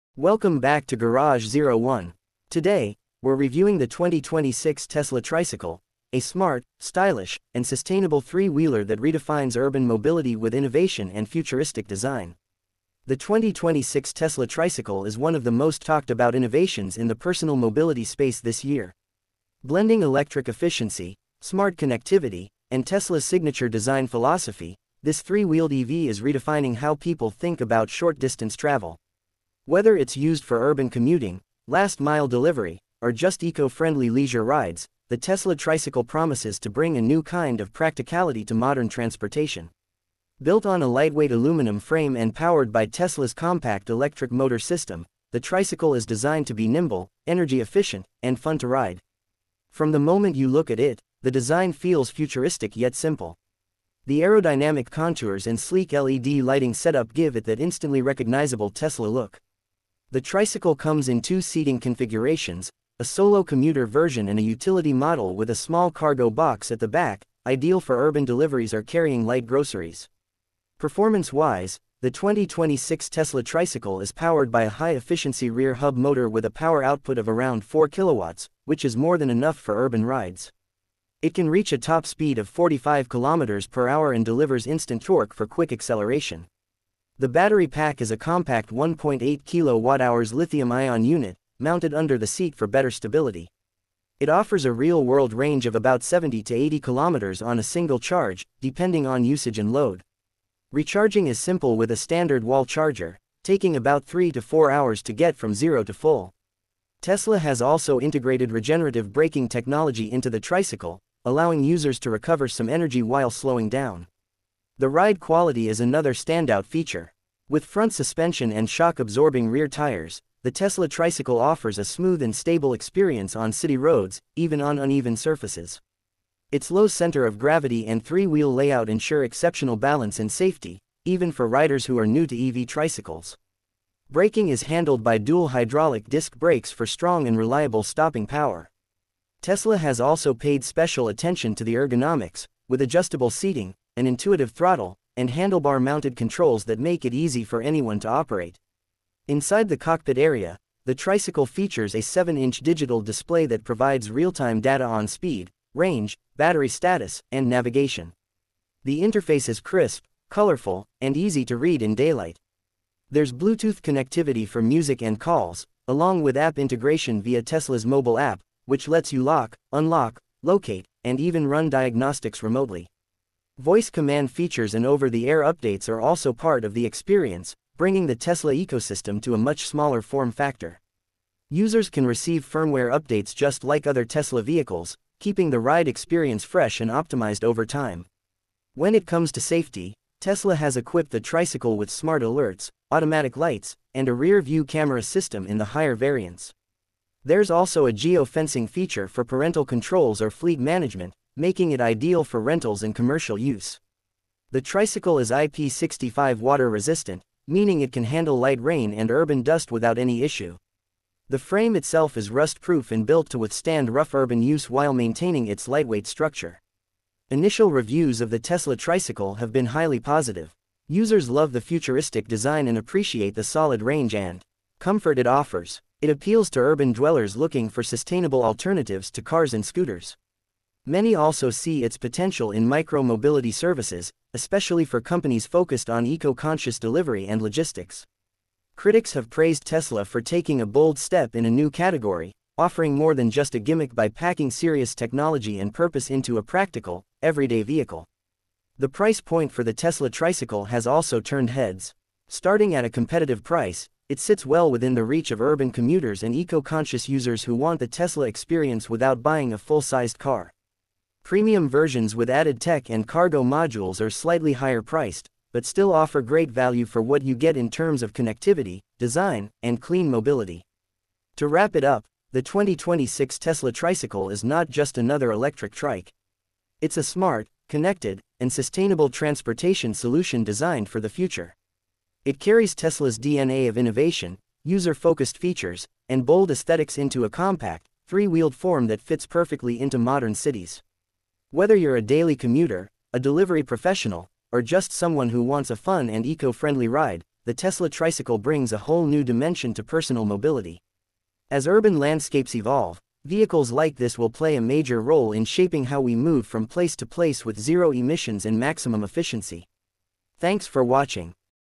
2026 Tesla Tricycle Is Real — And It's INSANE!
The 2026 Tesla Tricycle is Tesla's bold new take on urban mobility — a sleek, all-electric three-wheeler built for city life. Combining futuristic design with smart tech, it redefines what a personal EV can be.

🚌 Voiceover Only | 🎥 Full Specs Breakdown | 🛠️ Technical Review | 🌍 Urban Mobility Insights

Looking to learn more about the future of urban transport? This video dives deep into the 2026 Tesla Tricycle, featuring expert analysis, key specs, and real-world applications.

⏱️ Timestamps:

00:00 - Intro
00:25 - Overview of [2026 Tesla tricycle]
01:15 - Engine & Transmission Details
02:10 - Suspension, Frame & Chassis Design
03:00 - Braking & Safety Technologies
04:10 - Driver Comfort & Dashboard Layout
05:00 - Global Impact & Final Verdict
06:50 - Outro

🚨 Introducing the 2026 Tesla Tricycle – The Future of Personal Urban Mobility! 🚨

Tesla has done it again! In this video, we take a first look at the all-new 2026 Tesla Tricycle – a sleek, fully electric three-wheeler designed for city life, eco-conscious commuters, and tech lovers alike. With advanced AI features, impressive range, and a futuristic design, this trike might just be the next big thing in electric mobility.

🔋 Key Features Covered:

Top speed and range

Autonomous driving capabilities

Battery and charging specs

Price and release expectations

Real-world use cases & comparisons

Is this the vehicle that will reshape how we move through cities?